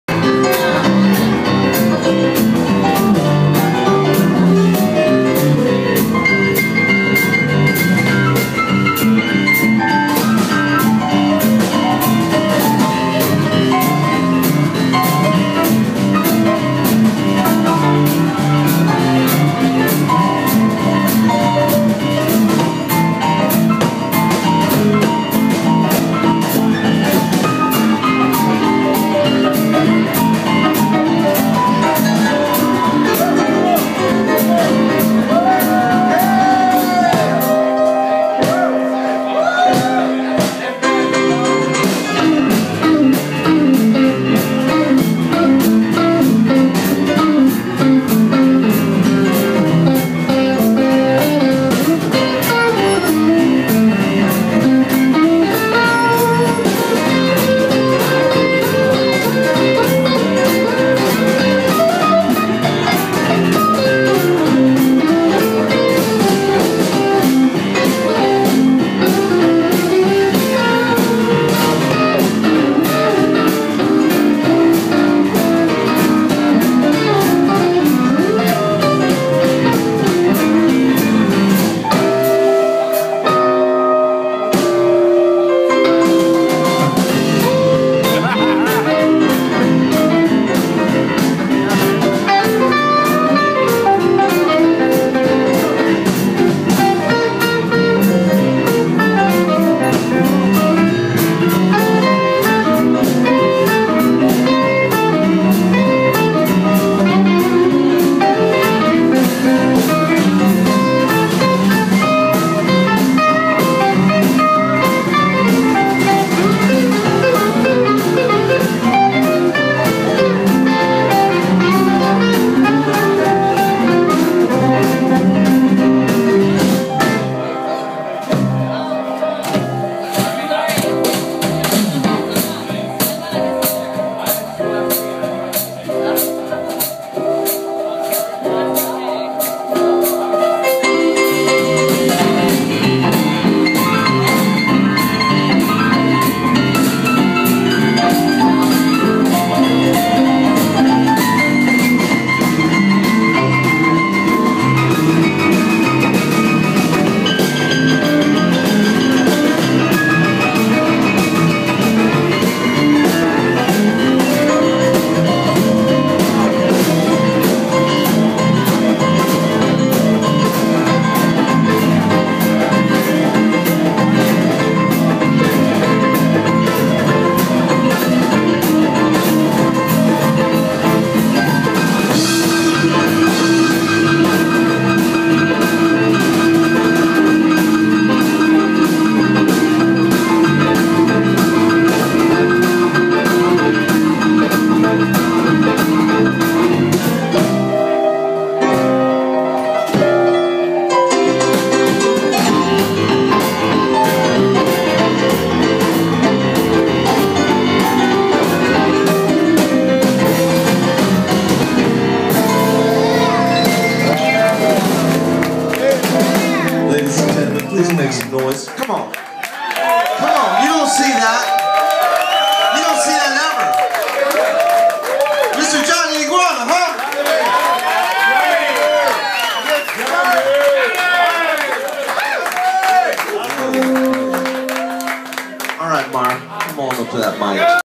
The Special 20s with Johnny Iguana
The Special 20s featuring Johnny Iguana, Mark Wydra, and Shoji Naito.
Beale on Broadway in St. Louis.